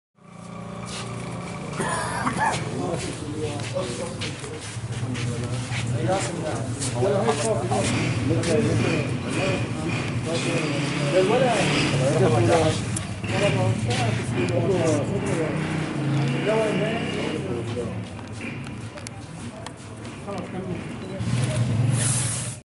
Rif Dimashq: Man farewell his brother killed due to the shelling over Erbin city 20-3-2017
This man fare welling his brother who was killed due to the Syrian regime shelling over Erbin city
رجل يودع شقيقه الذي قتل بقصف النظام على مدينة عربين بريف دمشق